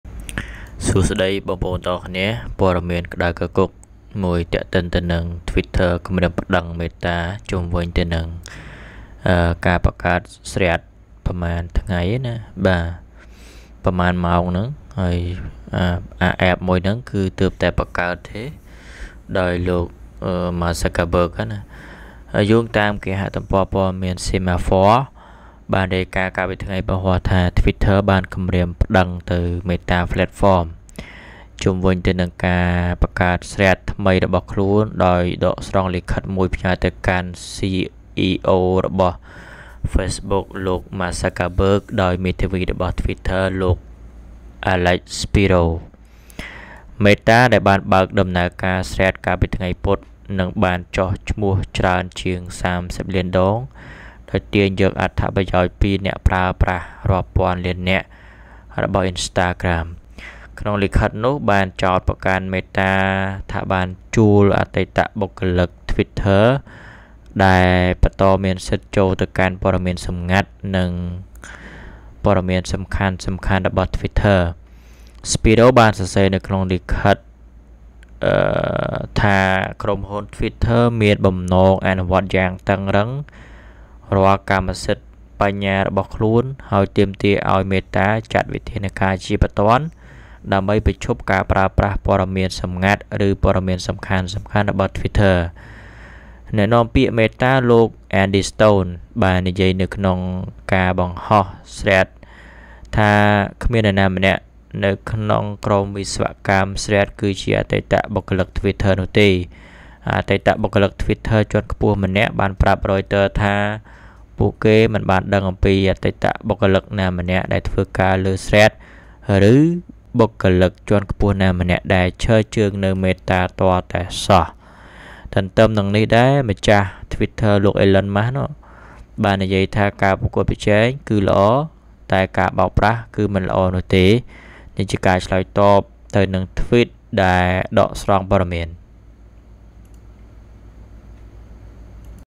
Twitter គំរាមប្តឹង Meta ជុំវិញ Threads | Rathy OnNews
Twitter គំរាមប្តឹង Meta ជុំវិញ Threads(Reuters)
គេហទំព័រព័ត៌មាន Semafor បានរាយការណ៍កាលពីថ្ងៃព្រហស្បតិ៍ថា Twitter បានគំរាមប្តឹង Meta Platforms (META.O) ជុំវិញ Threads ថ្មីរបស់ខ្លួន ដោយដកស្រង់លិខិតមួយផ្ញើទៅកាន់ CEO របស់ Facebook លោក Mark Zuckerberg ដោយមេធាវីរបស់ Twitter លោក Alex Spiro ។

Meta ដែលបានបើកដំណើរការ Threads កាលពីថ្ងៃពុធ និងបានចុះឈ្មោះច្រើនជាង 30 លានដង ដោយទាញយកអត្ថប្រយោជន៍ពីអ្នកប្រើប្រាស់រាប់ពាន់លាននាក់របស់ Instagram ។

នៅក្នុងលិខិតនោះបានចោទប្រកាន់ Meta ថាបានជួលអតីតបុគ្គលិក Twitter ដែល " បន្តមានសិទ្ធិចូលទៅកាន់ពត៌មានសម្ងាត់និងសំខាន់ៗរបស់ Twitter " ។

Spiro បានសរសេរនៅក្នុងសំបុត្រថា "Twitter មានបំណងអនុវត្តយ៉ាងតឹងរ៉ឹងនូវកម្មសិទ្ធិបញ្ញារបស់ខ្លួន ហើយទាមទារឱ្យ Meta ចាត់វិធានការជាបន្ទាន់ដើម្បីបញ្ឈប់ការប្រើប្រាស់ពត៌មានសម្ងាត់ឬពត៌មានសំខាន់ៗ របស់Twiiter" ។

អ្នកនាំពាក្យ Meta លោក Andy Stone បាននិយាយនៅក្នុងការបង្ហោះ Threads ថា "គ្មាននរណាម្នាក់នៅក្នុងក្រុមវិស្វកម្ម Threads គឺជាអតីតបុគ្គលិក Twitter នោះទេ" ។

អតីតបុគ្គលិក Twitter ជាន់ខ្ពស់ម្នាក់បានប្រាប់ Reuters ថាពួកគេមិនបានដឹងអំពីអតីតបុគ្គលិកណាម្នាក់ដែលធ្វើការលើ Threads ឬបុគ្គលិកជាន់ខ្ពស់ណាម្នាក់ដែលបានឈរជើងនៅ Meta ទាល់តែសោះ។

ទន្ទឹមនឹងនេះដែរម្ចាស់ Twitter លោក Musk បាននិយាយថា "ការប្រកួតប្រជែងគឺល្អ ការបោកប្រាស់មិនមែនជា" ជាការឆ្លើយតបទៅនឹង tweet ដែលដកស្រង់ព័ត៌មាន។